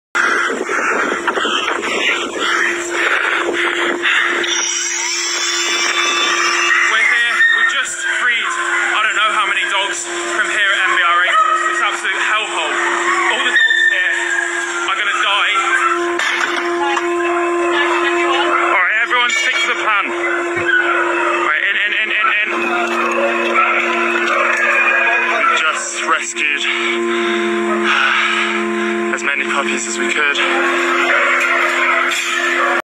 Animal Rising (formally Animal Rebellion) liberated 20 MBR Beagles in 2021!
The Animal Rising (formally Animal Rebellion) liberation of 20 MBR beagles on 20th December 2021. Two of them, Love and Libby were re-captured by the police and returned to MBR.